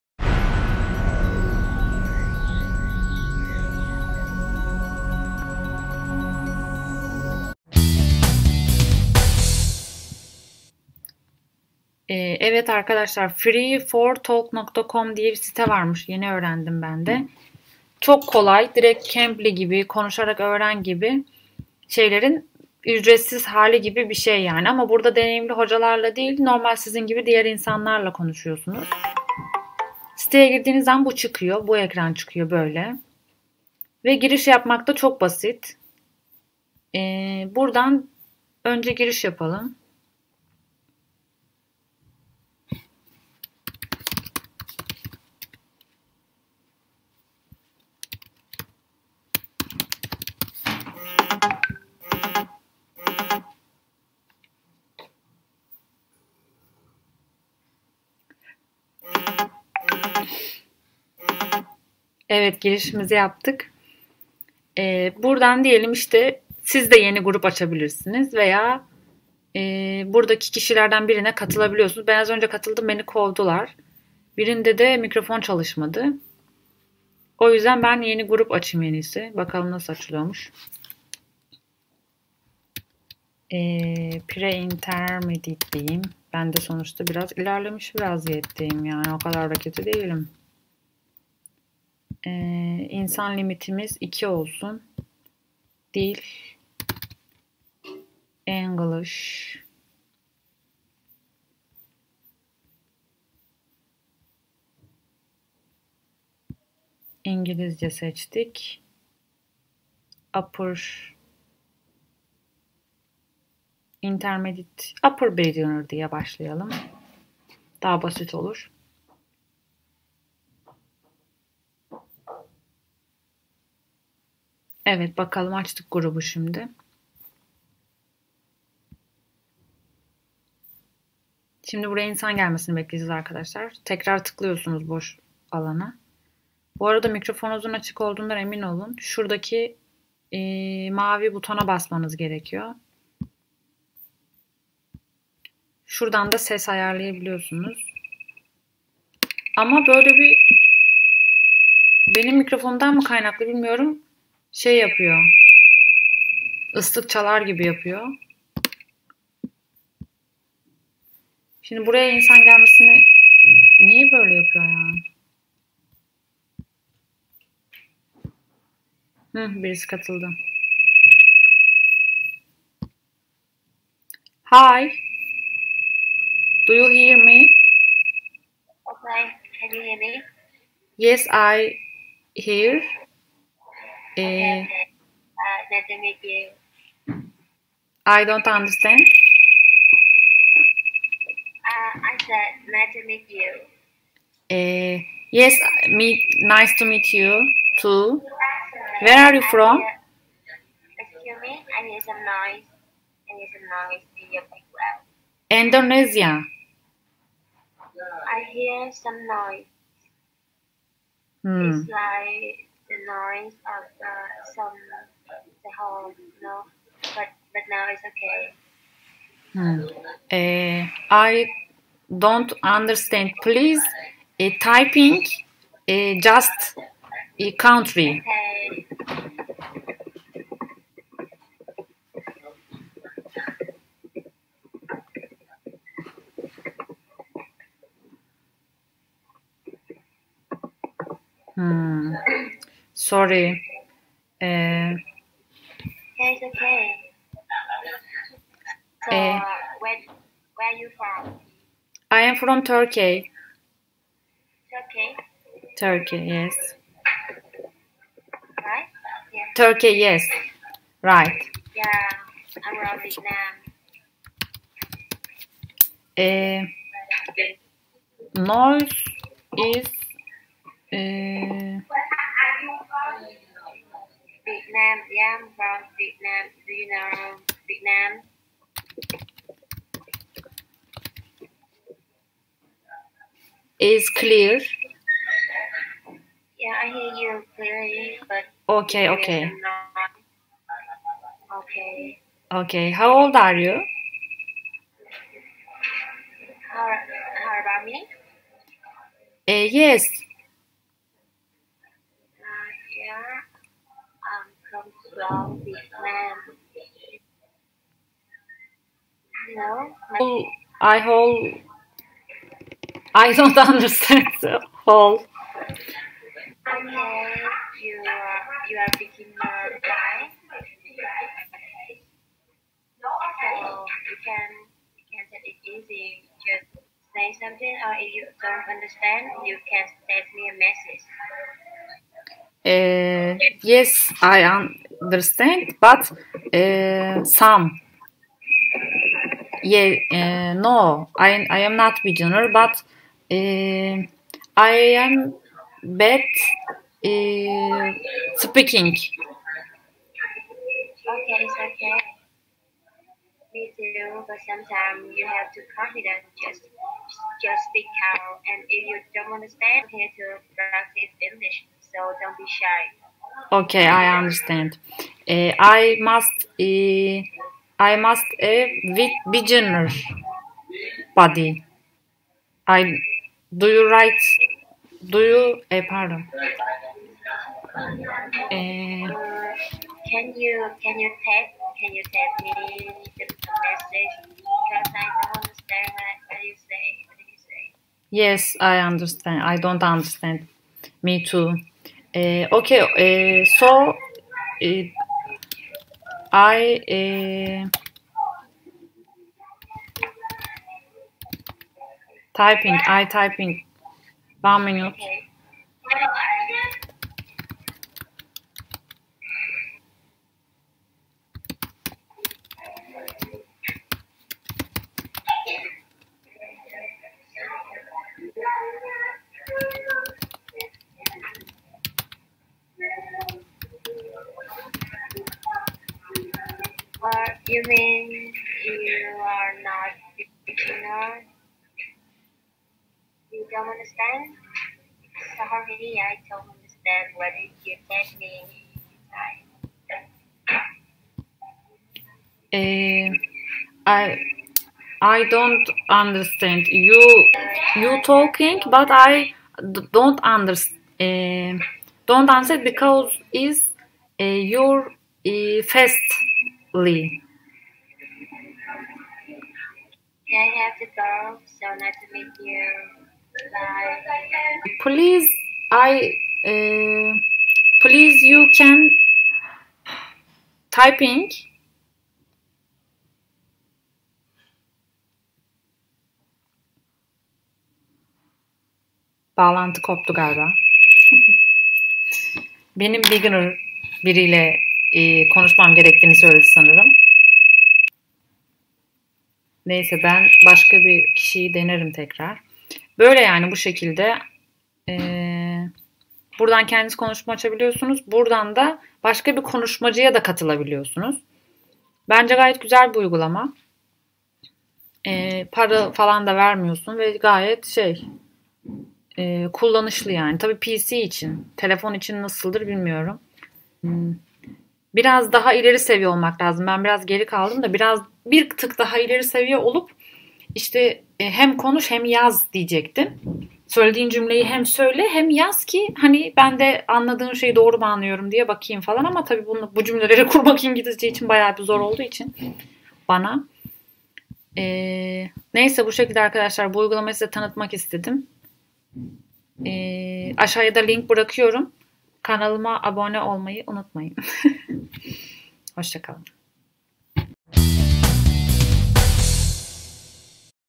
Cambly ve Konuşarak Öğren'in ÜCRETSİZ Versiyonu | FREE4TALK
Arkadaşlar merhaba, daha önce Cambly deneyimimi paylaşmıştım, yurtdışından hocalar tarafından görüntülü konuşabiliyor ve anlamadığınız kısımda size yazı ile destek oluyorlardı. Ancak çok pahalı ve sürekli pratik yapmamız gerekiyor.

Ücretsiz versiyon araştırmamda bir sonuca aslında ulaşamadım :D Ancak takip ettiğim ingilizce onlineingilizce öğren destek sayfalarından bu siteyi paylaştılar geçen gün. Denedim ve gerçekten tam aradığım Camly tadında ve kafamda kurduğum tarzda bir proje

Kesinlikle online ingilizce öğrenmek istiyorsanız girip denemenizi tavsiye ederim.
Buradaki tek handikap: Cambly'nin size sunduğu şeyleri burada sizin yaratmanız gerekiyor. Örneğin, anlamadığınız cümleleri karşı tarafın yazmasını talep edin. Yine anlamazsanız hemen online çevirin. Zaten böyle zorluklarla karşılaşmak daha güzel oluyor çünkü mücadele veriyorsunuz o cümleyi ingilizce kullanmak için.

Teşekkürler